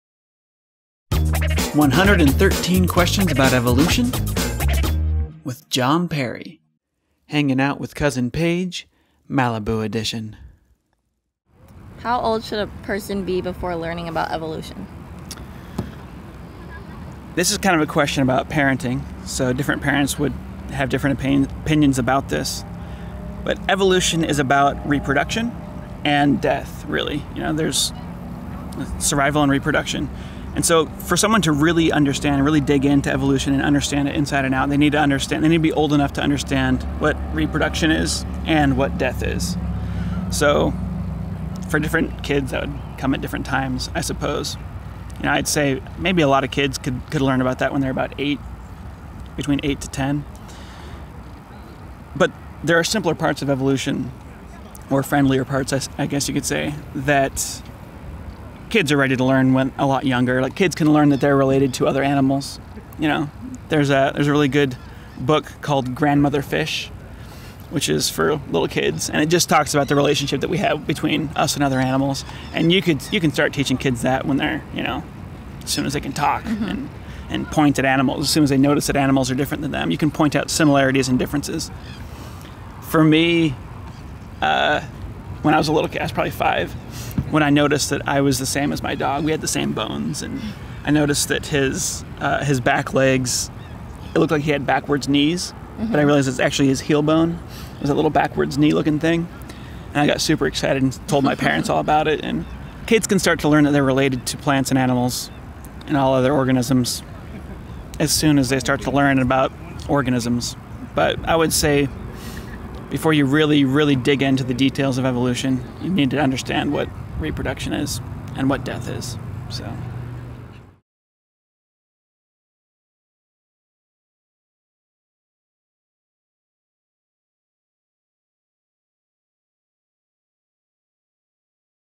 How old should kids be before learning evolution? Evolutionary Question #10
In this video I talk about some of the difficulties kids might have when they first learn about evolution and I talk about a great book recently created for kids called Grandmother Fish. I highly recommend it. I wish I had written it but I didn't :)